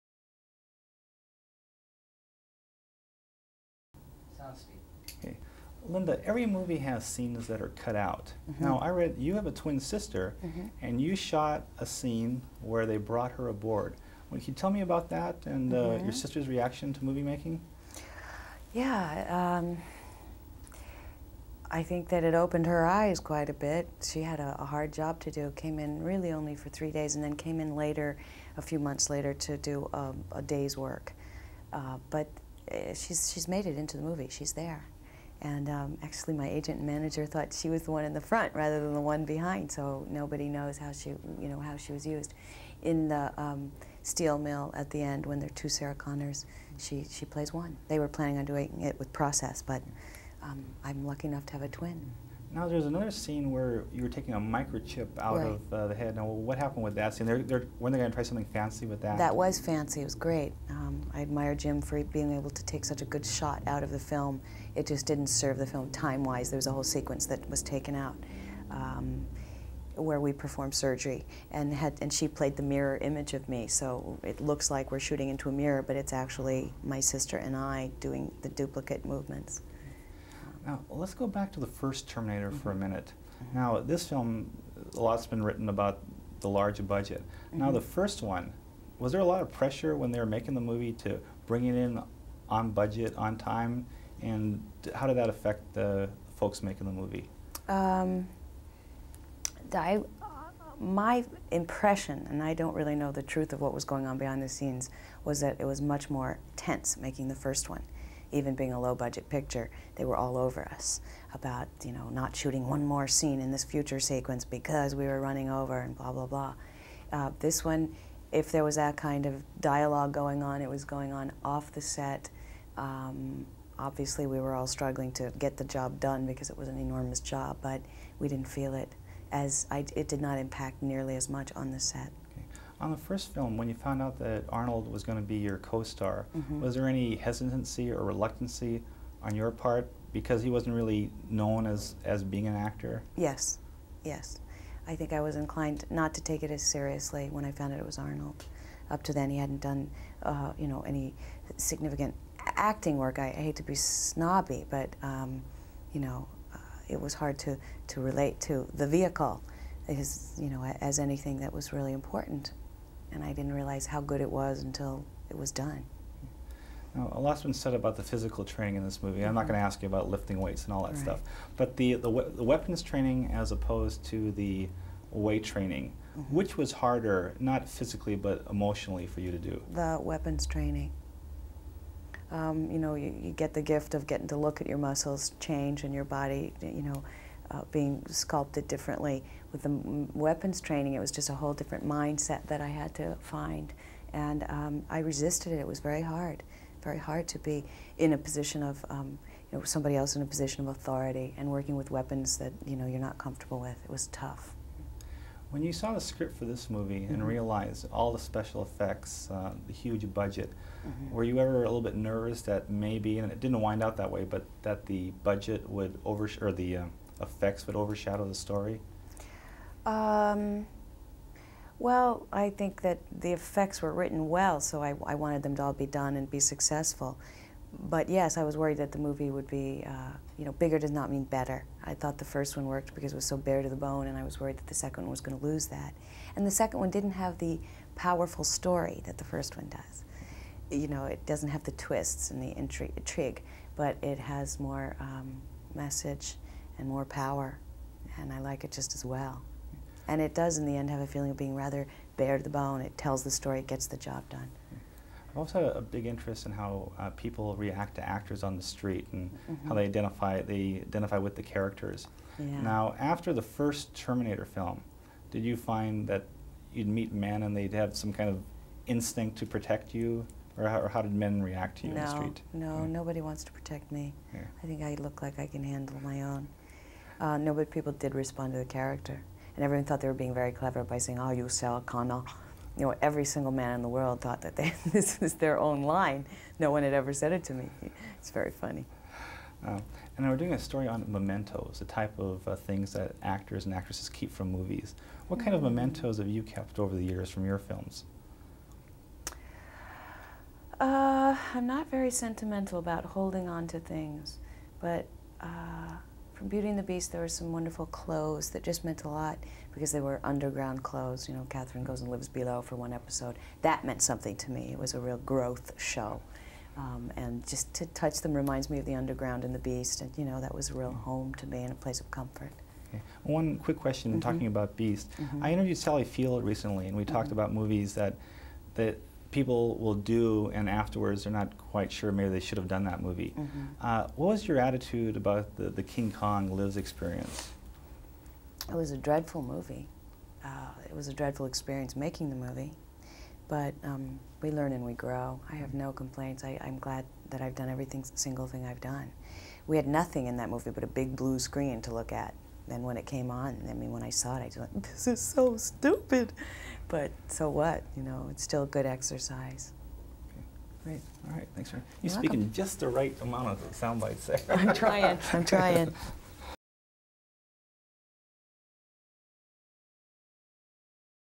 Rewind: Linda Hamilton on "Terminator 2." NEVER ask: "Are you Sarah Connor?"
My 1991 chat with Linda Hamilton for "Terminator 2." She discusses using her twin sister for the movie, how she wasn't pleased when Schwarzenegger was cast for the first "Terminator," how people ask, "Are you Sarah Connor?" And much more.